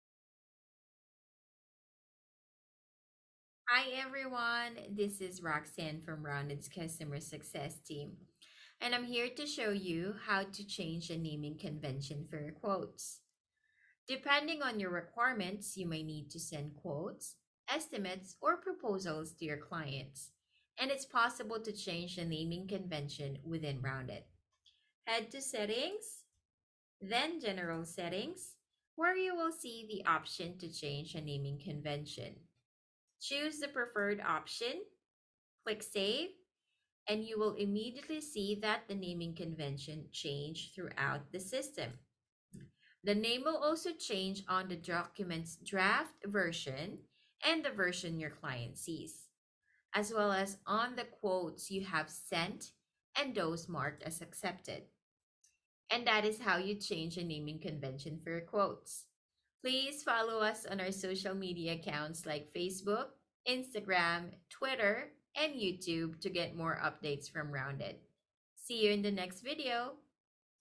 How to change the naming convention of Quotes
Depending on the kind of work you do, you may need to send quotes, estimates or proposals. And its possible to change the naming convention within Rounded, and this video will show you how.